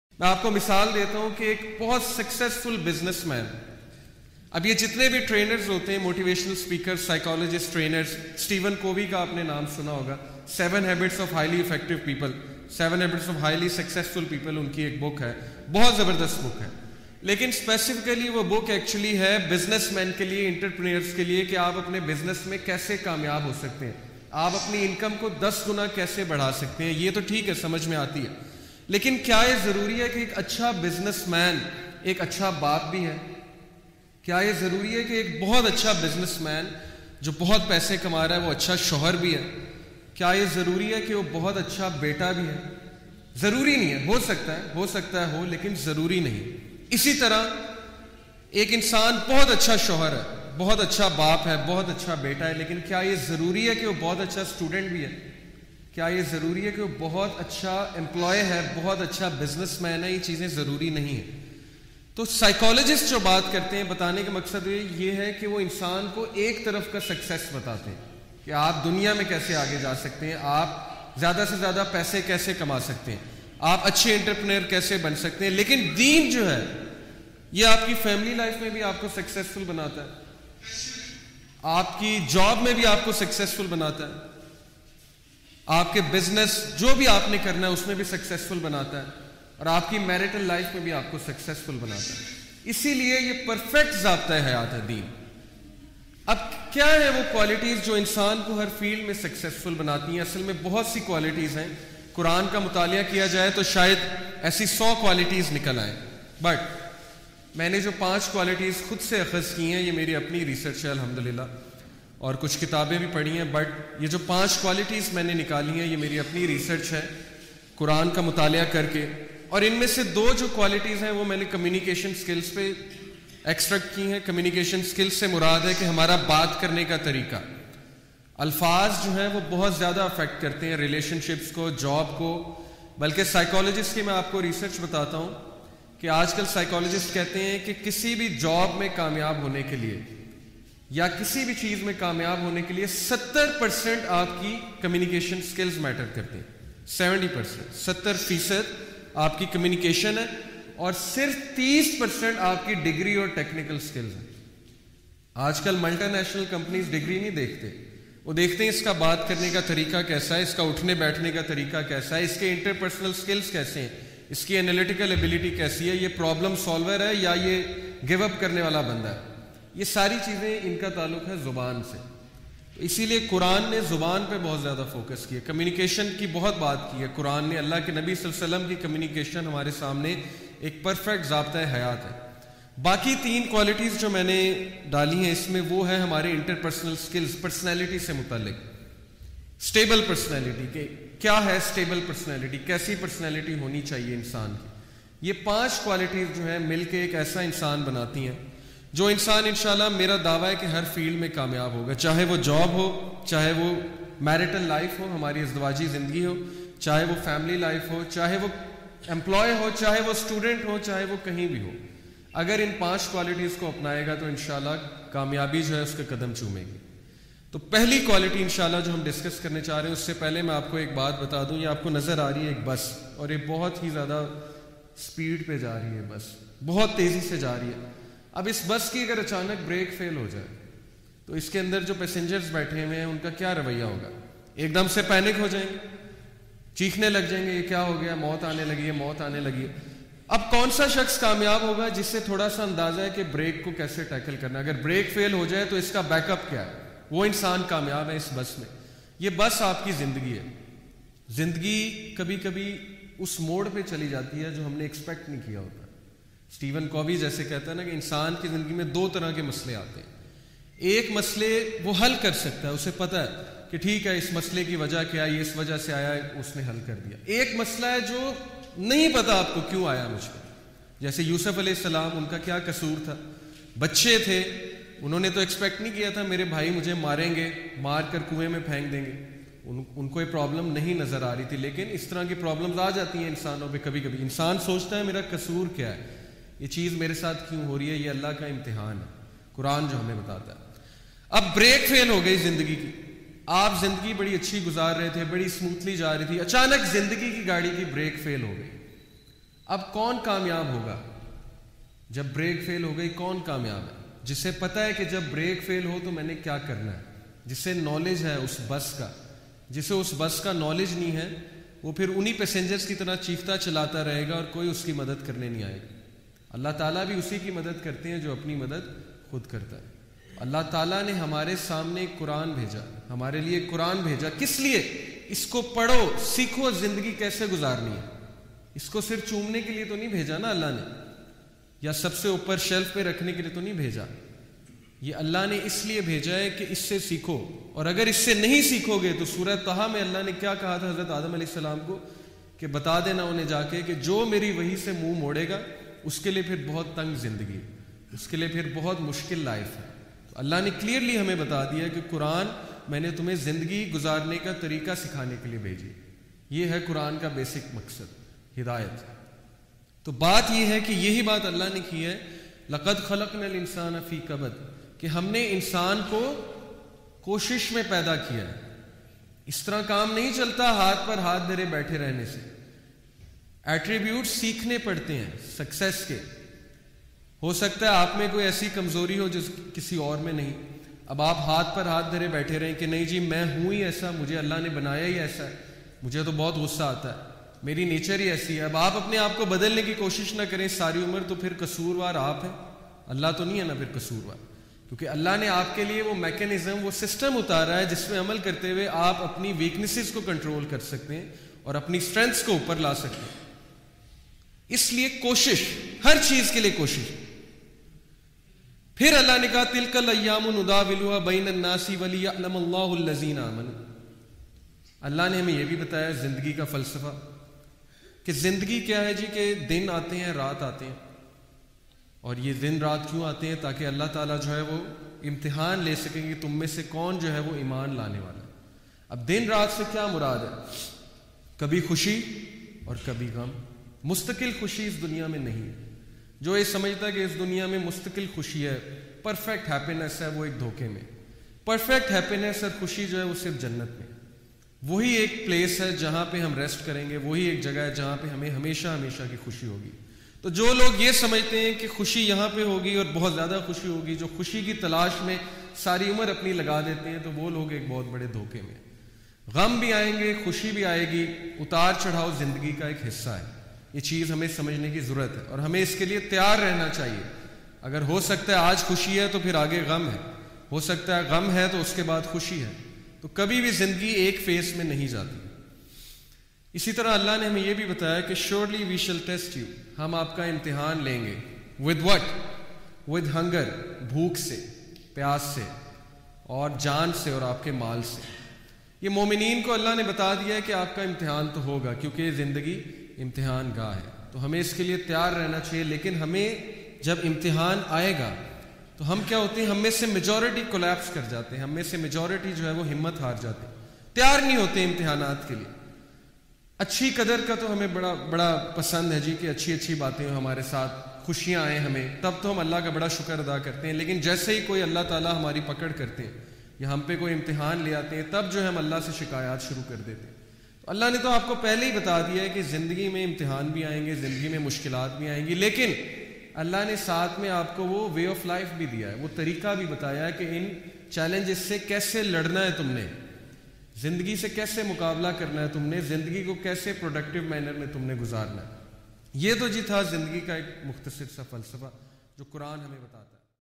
If You Want To Succeed In Life, Never Give Up || Life Changing Reminder By Muhammad Ali | YouthClub
Topic: If You Want To Succeed In Life, Never Give Up
Video Credit To -- Muhammad Ali

▶Use Headphones🎧